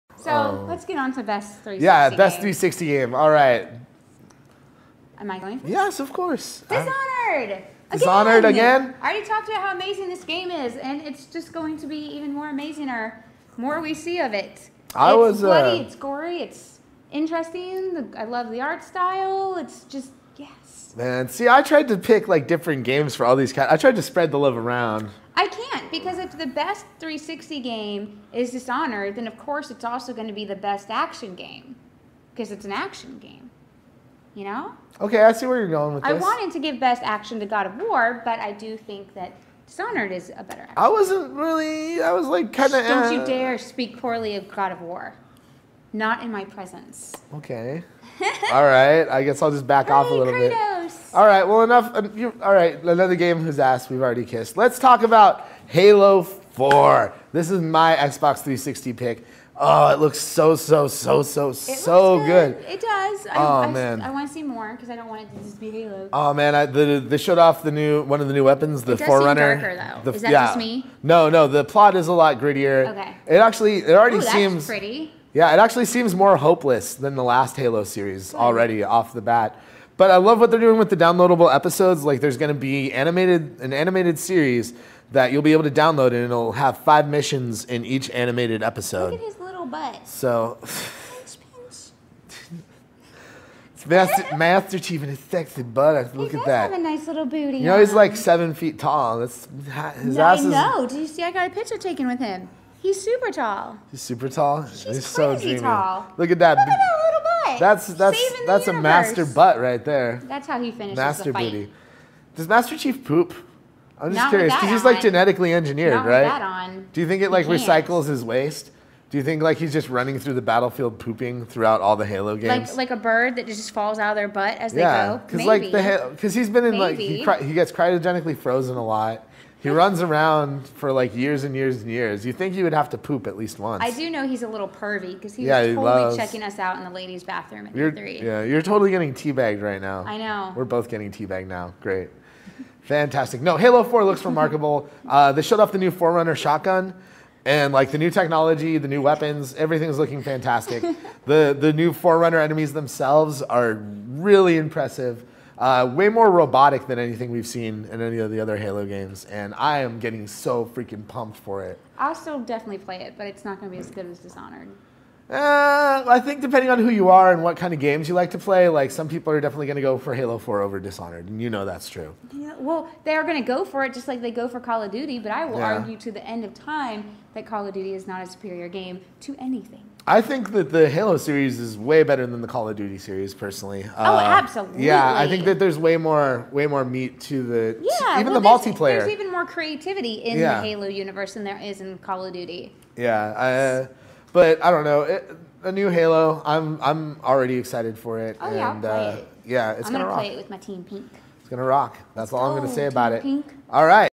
Best Xbox 360 Games - Dishonored and Halo 4: Week in Geek, Episode 21
Xbox 360: Raychul's pick - Dishonored for bloody and gory and action too! Blake's - Halo 4 huh huh, his butt pack looks like a ninja turtle -- Watch full episodes and tune in Live every Friday at Noon PST.